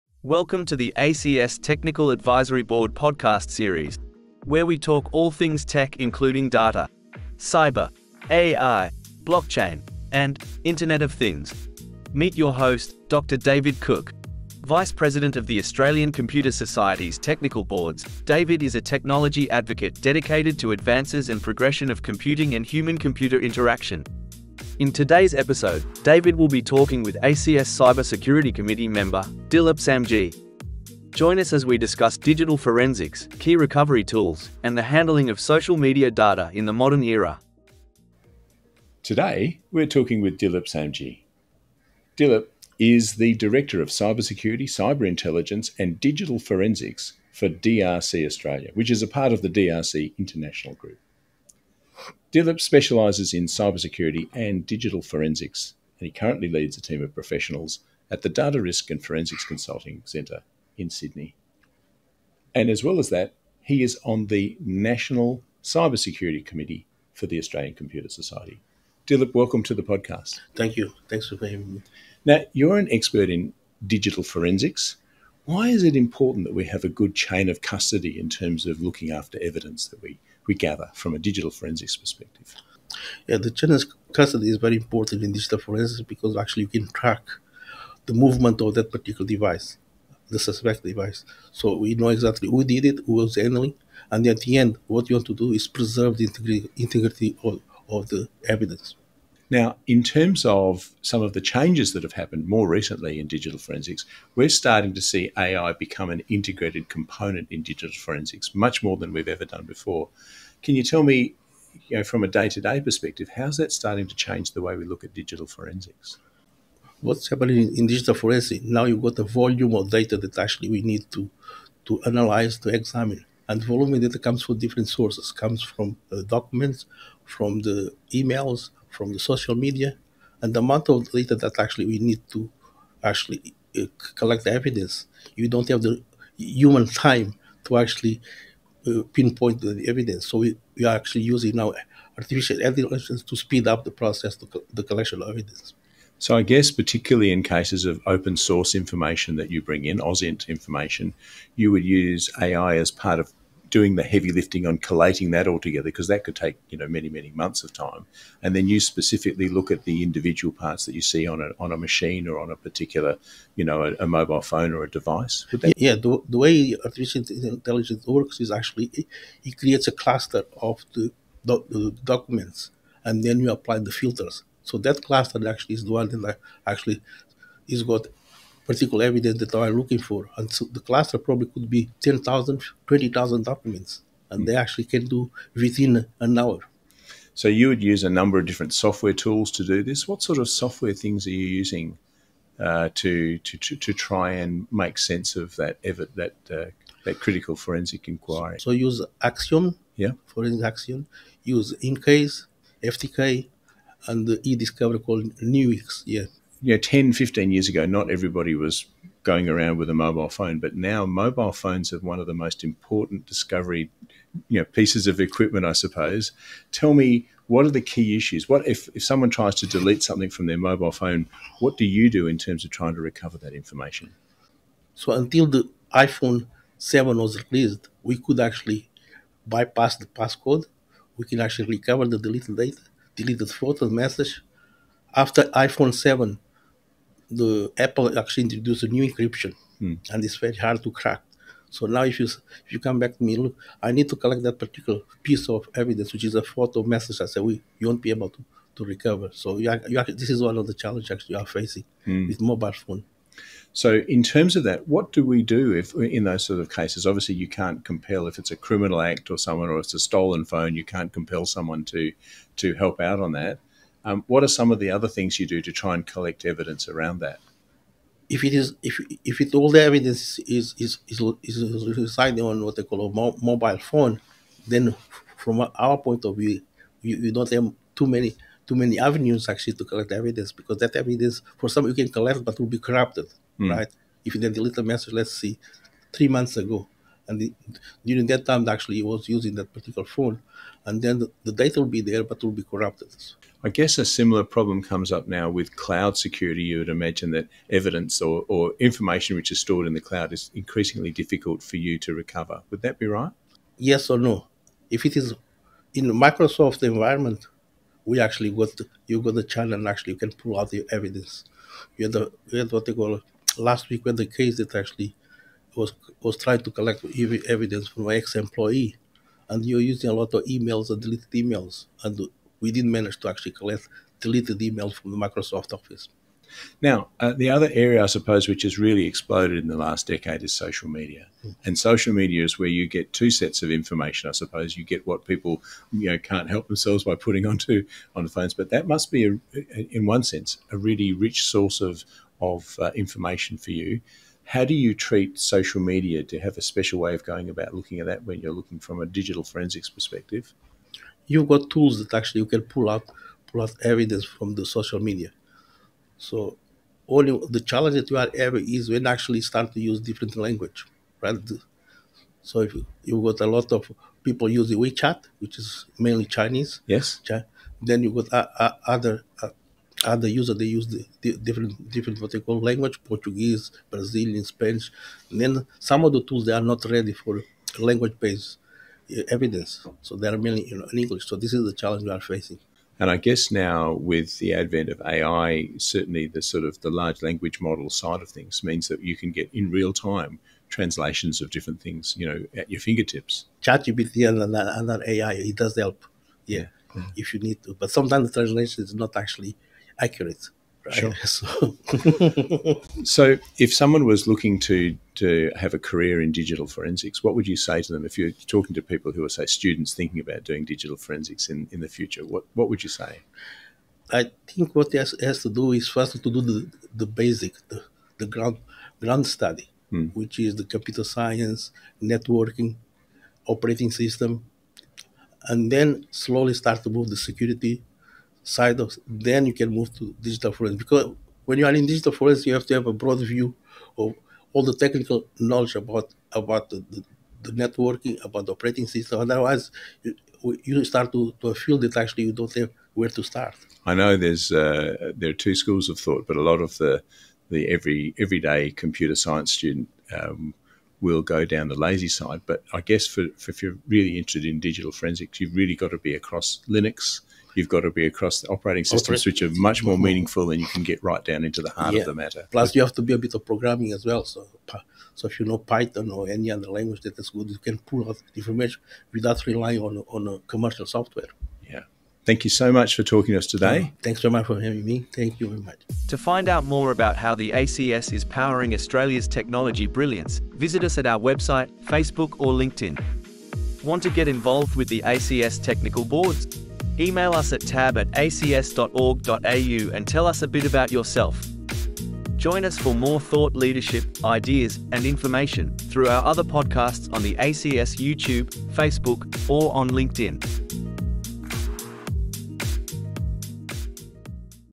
Episode 21 - Cyber Security & Digital Forensics
In this Podcast, Dr David Cook talks with ACS Cyber Security committee member, Dilip Samji. Join us as they discuss digital forensics, key recovery tools and the handling of social media data in the modern era.

Guest: Dilip Samji – Cyber Security Committee Member
Host: Dr David Cook, VP – Academic Boards